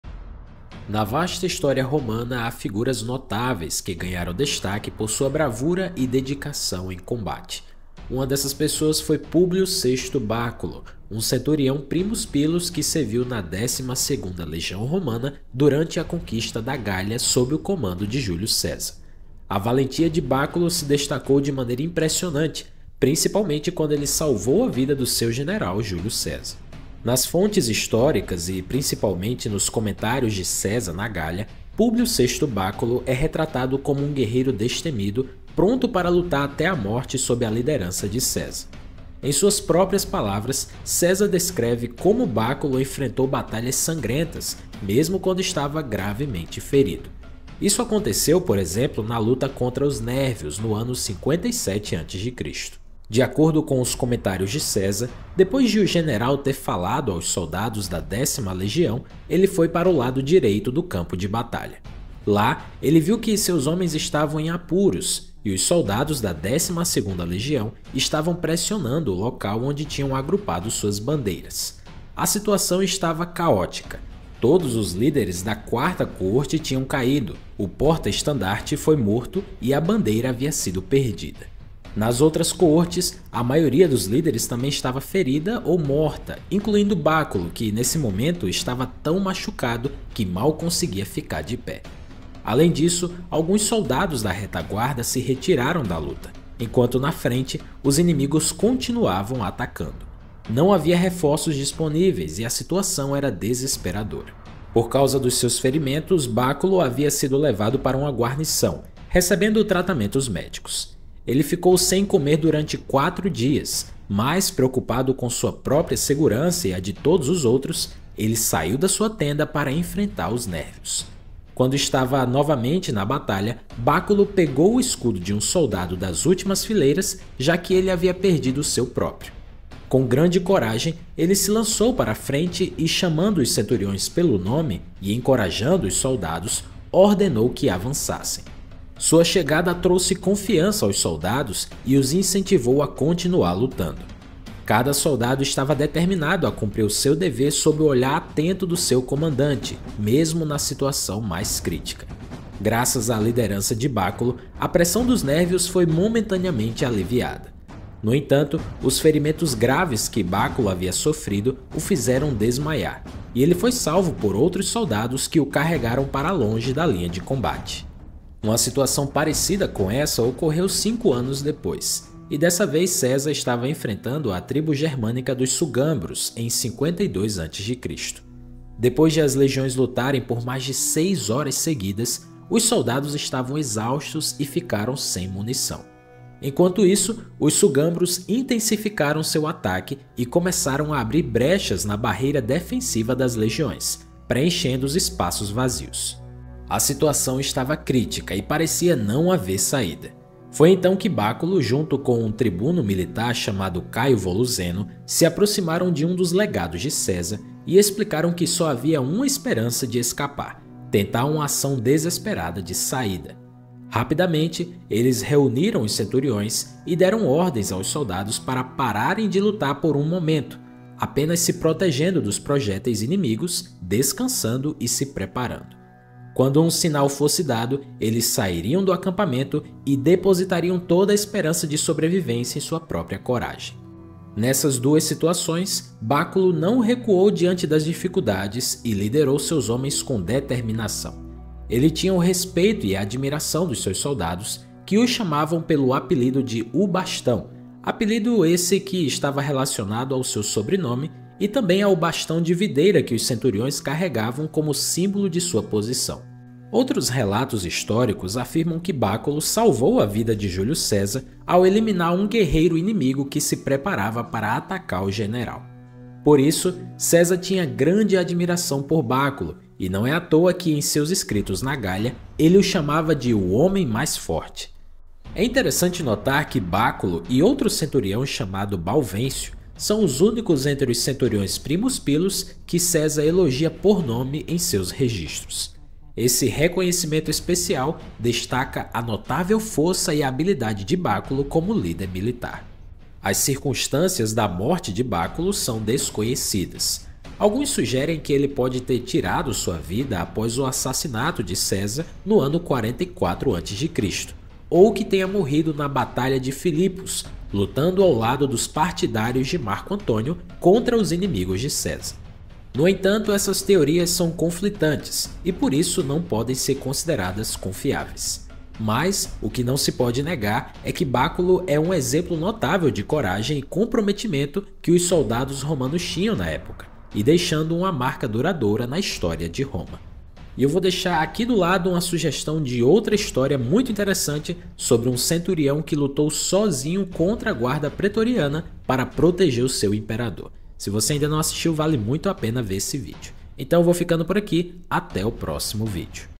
O SOLDADO ROMANO MAIS FORTE DAS LEGIÕES DE JÚLIO CÉSAR
Na vasta história romana, há figuras notáveis que ganharam destaque por sua bravura e dedicação em serviço. Uma dessas pessoas foi Publius Sextius Baculus, um centurião Primus pilus que serviu na XII legião romana durante a conquista da Gália, sob o comando de César. A valentia de Baculus se destacou de maneira impressionante, principalmente quando ele salvou a vida de seu general, Júlio César.

📚 Bons livros sobre História Antiga